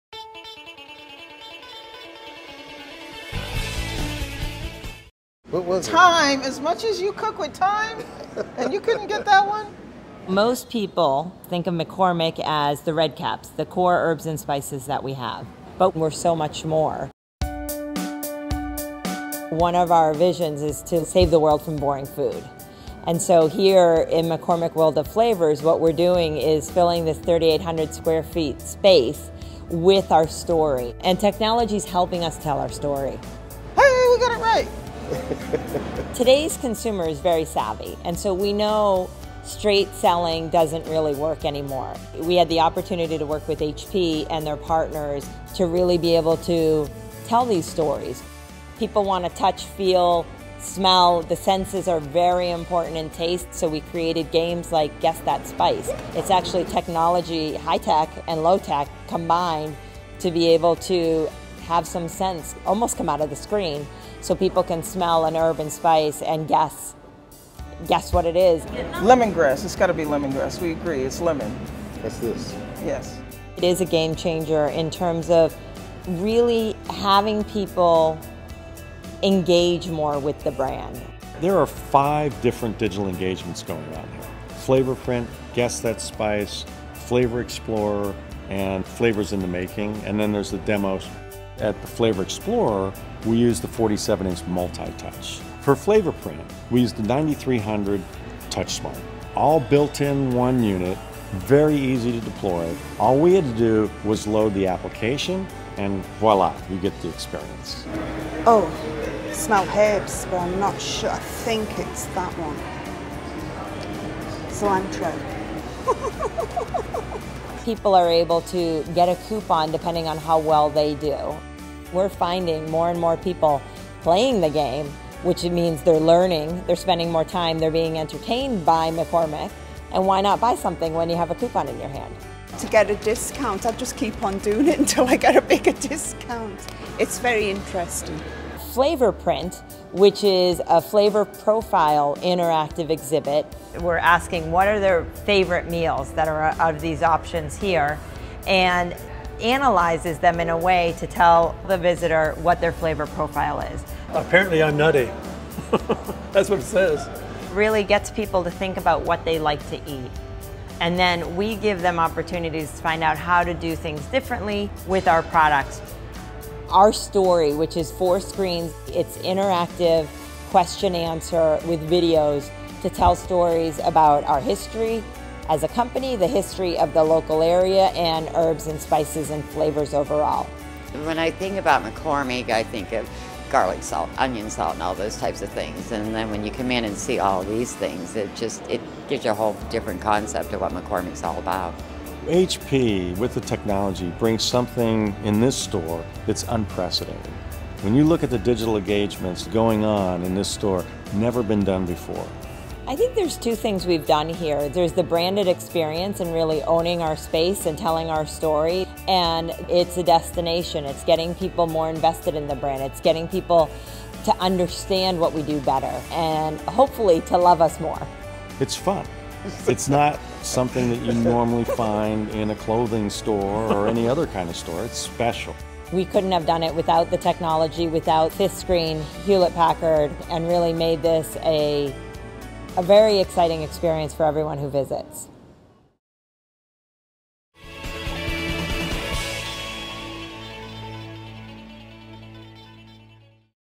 Some of our early work "Guess that Spice" McCormick World of Flavors
5th Screen developed 5 digital engagments for the McCormick World of Flavors Store